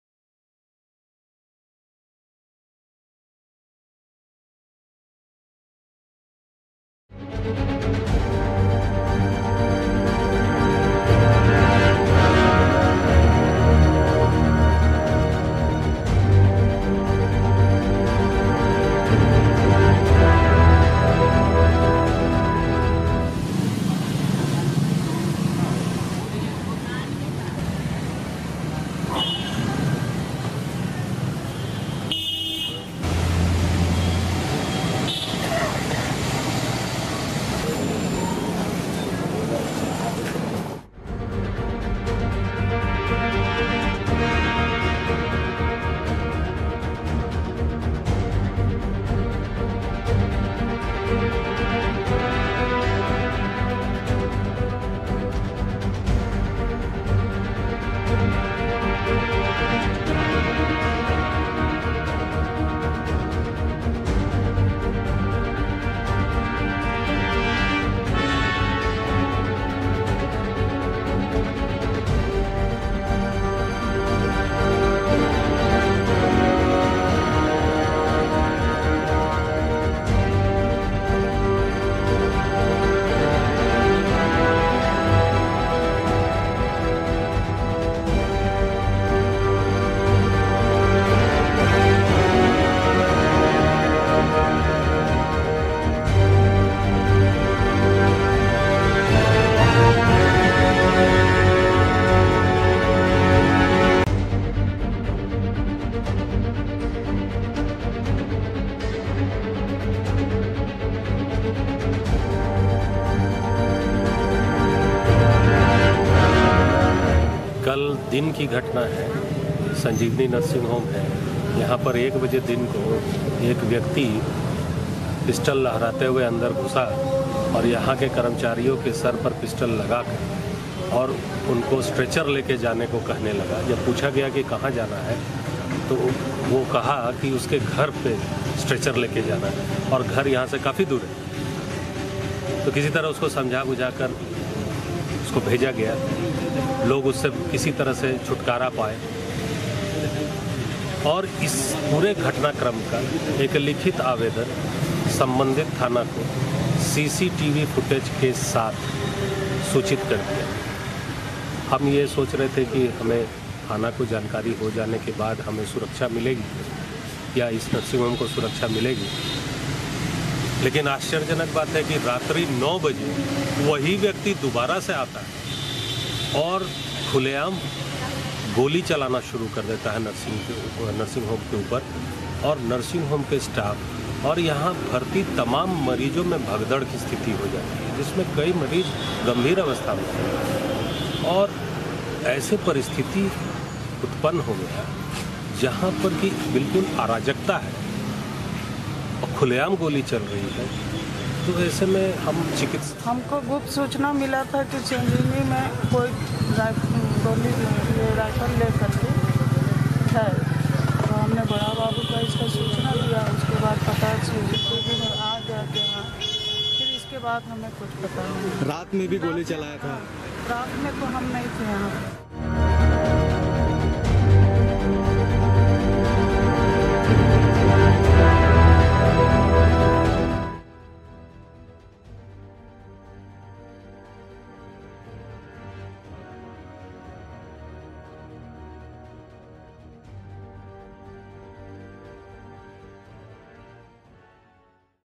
Dhanbad के संजीवनी नर्सिंग होम में बंदूक लहराता हुआ पहुंचा शख्स
झारखंड के धनबाद में एक शख्स अस्पताल लेकर पहुंचा पिस्टल... संजीवनी नर्सिंग होम में बंदूक लहराता हुआ पहुंचा शख्स... नर्सिंग होम पर तीन से चार राउंड गोली चलाकर फैला आतंक...